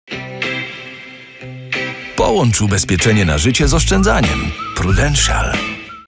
Młodzi Bogowie 2w1 dla pewnej przyszłości – reklama Prudential 6s
Ubezpiecz się zdalnie! Szczegóły u naszego Konsultanta. Ubezpieczenie na życie? A może oszczędzanie? W Prudential masz rozwiązanie 2w1 łączące ubezpieczenie na życie z oszczędzaniem na dowolny cel. Nie musisz się już dwoić!